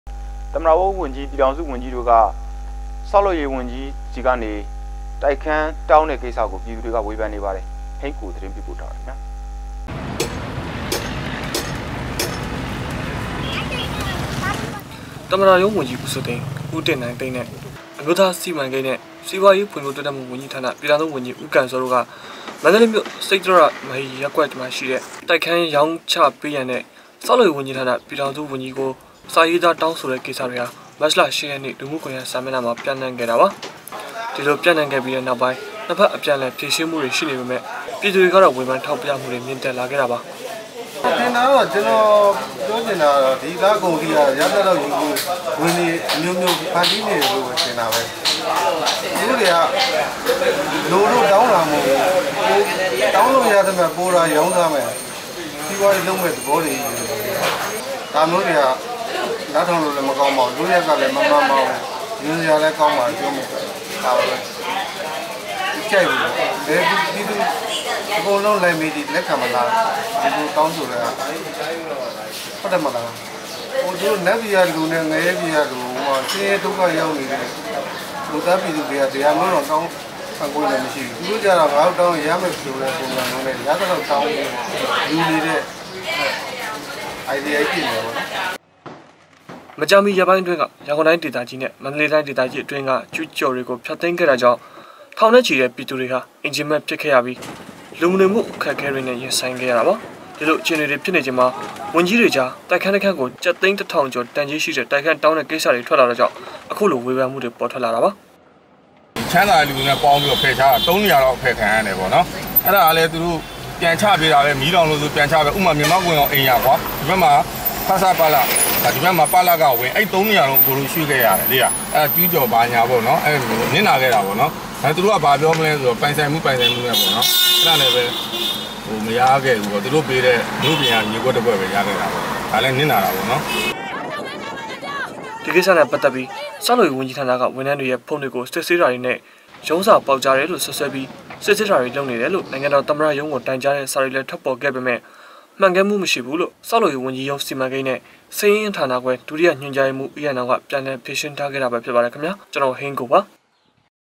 DVB TV - ၀န္ၾကီးတုိက္ခန္းေတာင္းတဲ့အေပၚျပည္သူေ၀ဖန္မႈရွိ
DVB TV - သမၼတ႐ုံး၀န္ႀကီး ၊ ျပည္ေထာင္စု၀န္ႀကီးတို႔ကေဆာက္လုပ္ေရး၀န္ၾကီးဌာနျပည္ေထာင္စု၀န္ၾကီးဆီကိုတိုက္ခန္းေတာင္းတဲ့အေပၚျပည္သူေတြကေ၀ဖန္မႈေတြရွိေနပါတယ္။ဟိန္းကိုသတင္းေပးပို႔ထားတာပါ။